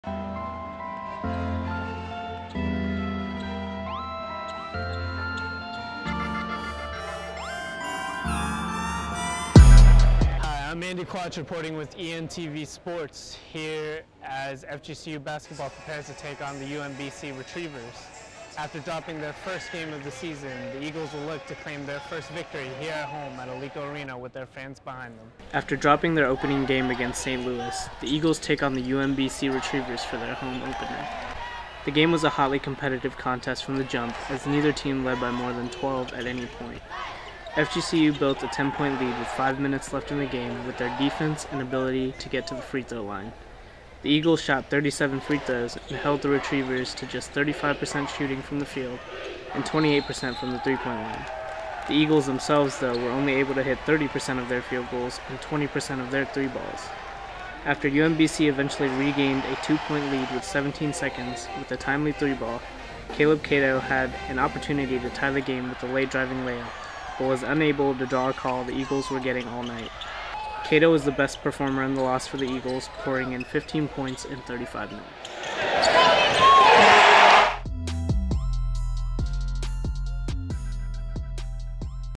FGCU Men's Basketball Home Opener
Highlights and recap from FGCU's home opener against UMBC

Producer: Andy Quach
Reporter: Andy Quach
Editor: Andy Quach
Camera: Andy Quach/Christian Sanchez